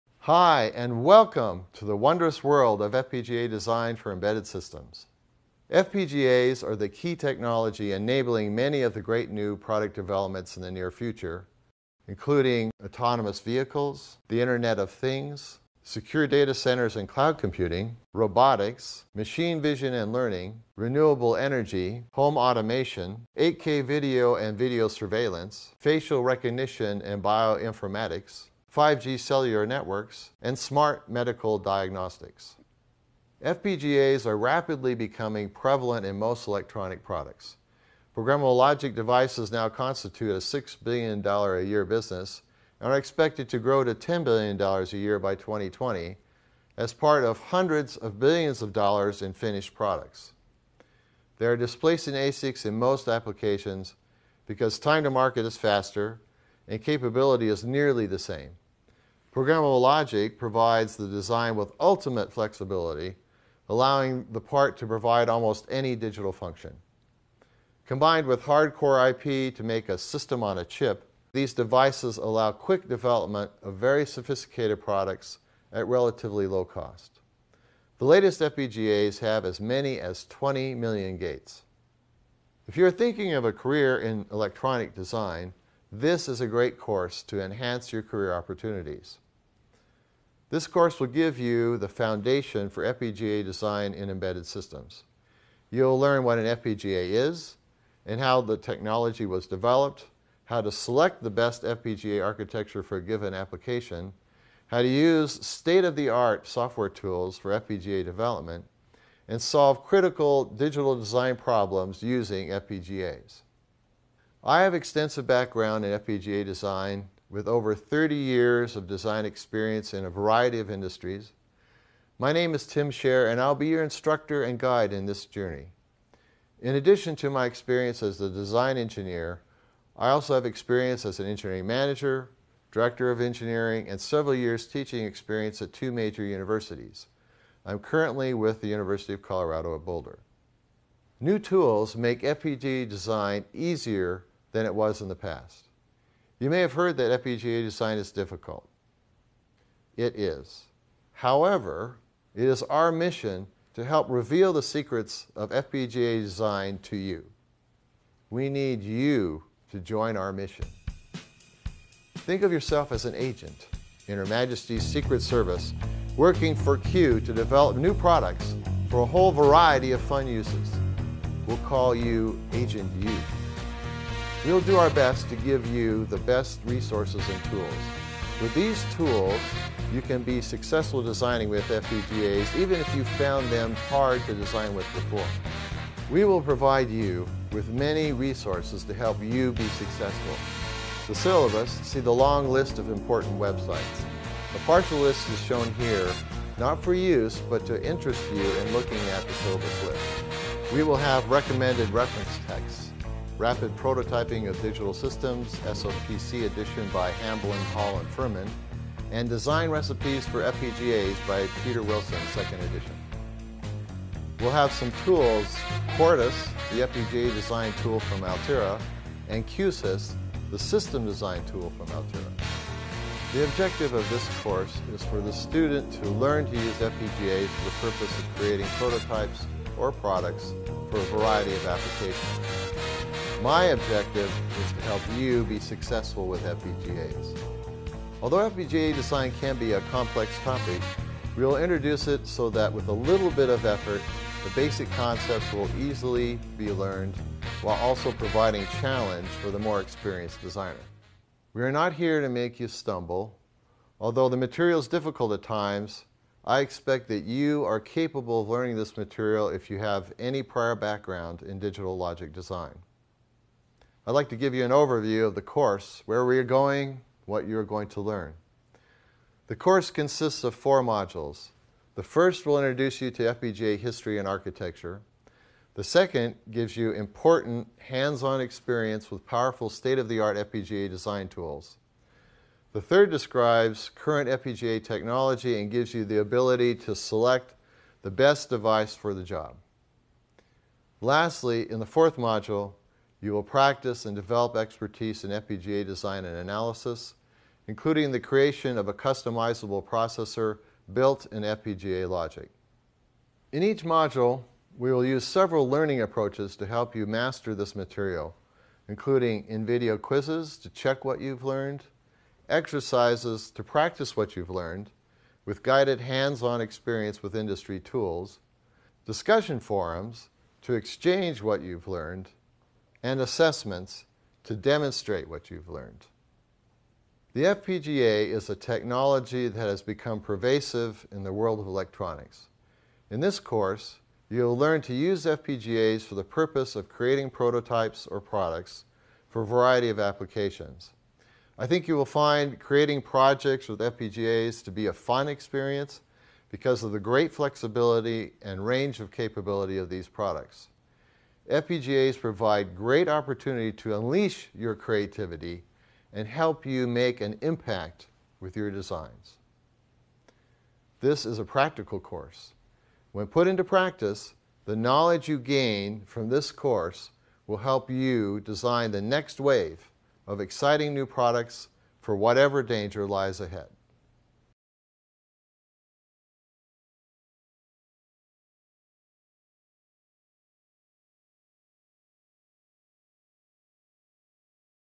Course Overview
Hi, and welcome to the wondrous world of FPGA design for embedded systems. FPGAs are the key technology enabling many of the great new product developments in the near future. Including autonomous vehicles, the Internet of Things, secure data centers and cloud computing, robotics, machine vision and learning, renewable energy, home automation, 8K video and video surveillance, facial recognition and bioinformatics, 5G cellular networks, and smart medical diagnostics. FPGAs are rapidly becoming prevalent in most electronic products. Programmable logic devices now constitute a $6 billion a year business, and are expected to grow to $10 billion a year by 2020 as part of hundreds of billions of dollars in finished products. They are displacing ASICs in most applications because time in market is faster and capability is nearly the same. Programmable logic provides the design with ultimate flexibility, allowing the part to provide almost any digital function.

If you're thinking of a career in electronic design, this is a great course to enhance your career opportunities. This course will give you the foundation for FPGA design in embedded systems. You'll learn what an FPGA is and how the technology was developed. How to select the best FPGA architecture for a given application. How to use state of the art software tools for FPGA development. And solve critical digital design problems using FPGAs. I have extensive background in FPGA design, with over 30 years of design experience in a variety of industries. My name is Tim Scherr, and I'll be your instructor and guide in this journey. In addition to my experience as a Design Engineer, I also have experience as an Engineering manager, Director of Engineering and several years teaching experience at two major universities. I'm currently with the University of Colorado at Boulder. New tools make FPGA design easier than it was in the past.

You may heard that FPGA design is difficult. It is, however, it is our mission to help reveal the secrets of FPGA design to you.

We need you to join our mission. Think of yourself as an agent in Her Majesty's Secret Service, working for Q to develop new products for a whole variety of fun uses. We'll call you Agent You. We will do our best to give you the best resources and tools. With these tools you can be successful designing with FPGA even if you found them hard to design with before. We will provide you with many resources to help you be successful. The syllabus, see the long list of important websites. The partial list is showing here, not for use, but to interest you in looking at the silver switch. We will have recommended reference texts. Rapid Prototyping of Digital Systems, SOPC edition by Hamblen, Hall and Furman and Design Recipes for FPGAs by Peter Wilson, Second Edition.

We'll have some tools, Quartus the FPGA design tools from Altera ,and Qsys the system design tool from Altera. The objective of this course is for the student to learn these FPGAs for the purpose In creating prototypes or products for a variety of applications. My objective is to help you be successful with FPGAs. Although FPGA design can be a complex topic, we will introduce it so that with a little bit of effort, the basic concepts will easily be learned, while also providing challenge for the more experienced designer. We're not here to make you stumble. Although the material's difficult at times, I expect that you are capable of learning this material if you have any prior background in digital logic design. I'd like to give you an overview of the course, where we're going, what you're going to learn. The course consists of four modules. The first will introduce you to FPGA history and architecture. The second gives you important hands on experience with powerful state of the art FPGA design tools. The third describes current FPGA technology and gives you the ability to select the best device for the job. Lastly, in the fourth module, you will practice and develop expertise in FPGA design and analysis, including the creation of a customizable processor built in FPGA logic.